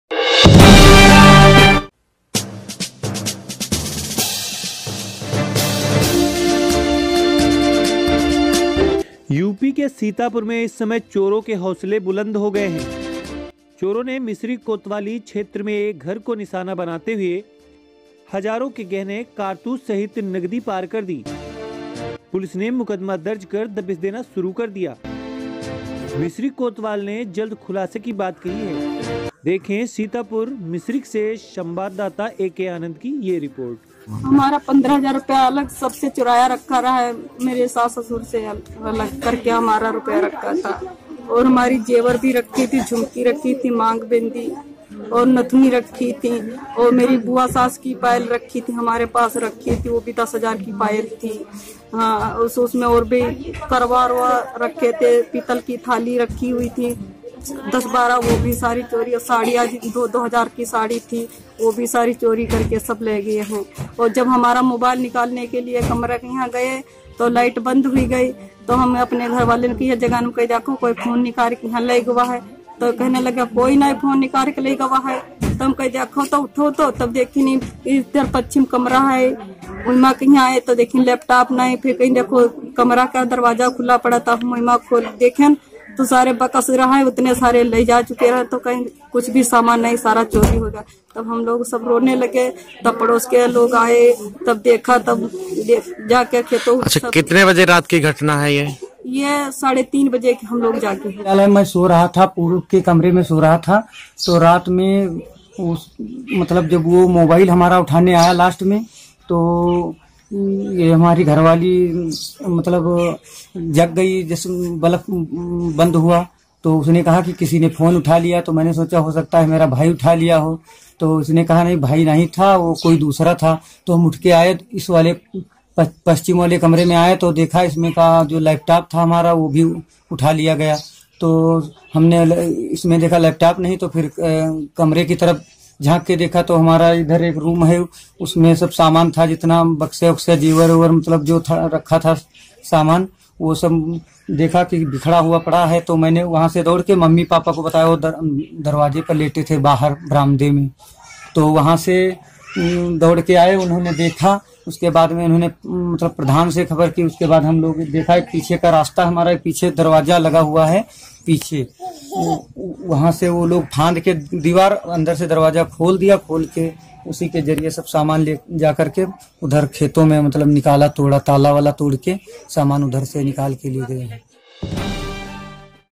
चोरों ने मिश्रिख कोतवाली क्षेत्र में एक घर को निशाना बनाते हुए हजारों के गहने ,कारतूस , नगदी चोरी
SharpMedia_सीतापुर में चोरों के हौसले बुलंद हो गए हैं।
चोरों ने मिश्रिख_कोतवाली क्षेत्र में एक घर को निशाना बनाते हुए हजारों के गहने ,कारतूस , सहित नगदी पार कर दी।पुलिस ने मुकदमा दर्ज कर दबिश देना शुरू कर दिया।
मिश्रिख कोतवाल ने जल्द खुलासे की बात कही है।

 देखें सीतापुर से धीरेन्द्र प्रताप सिंह के साथ संवाददाता ए. के आनंद की रिपोर्ट।